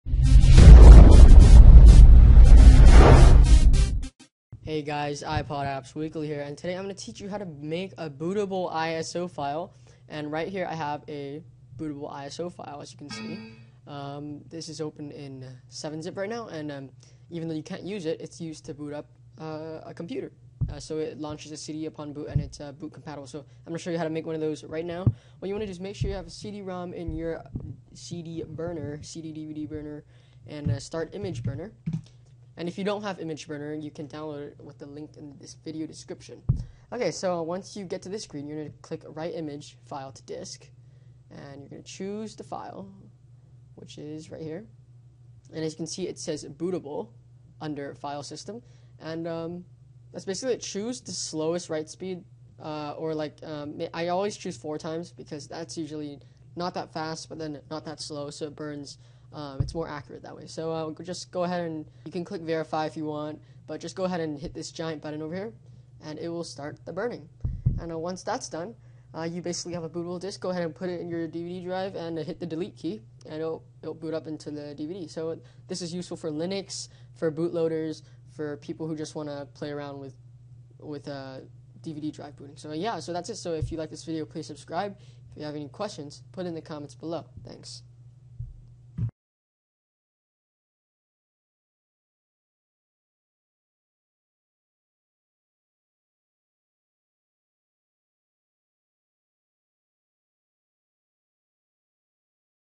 [HD] How to create a Bootable ISO CD DVD in Windows!
Boot from this CD! Easy Step by Step!